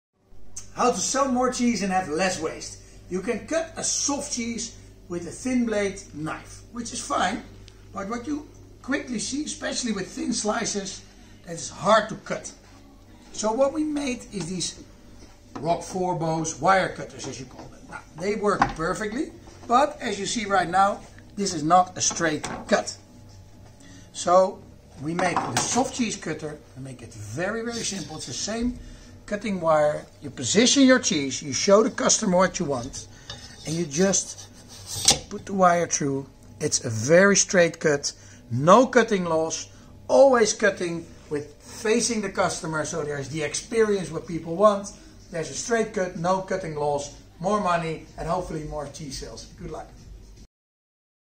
Boska Soft Cheese Cutter - 602020 (NL)
Met zachte hand zachte kazen snijden? Dat lukt je met deze ingenieuze kaassnijder. De snijdraad in de beugel glijdt door de kaas heen, je drukt deze dus niet plat en de kaas kan niet blijven plakken. De geleiders bieden ruimte om met een wiegende beweging te snijden, dus je kunt ook kazen snijden die net iets harder zijn dan bijvoorbeeld de zachte kaas Roquefort. Gaat perfect door de beugel.

Voordelen:
- Minimaal snijverlies
- Efficiënt snijden
- Onderhoudsvriendelijk,
 vaatwasmachine bestendig
- Veilig in gebruik

# 602020 Soft Cheese Cutter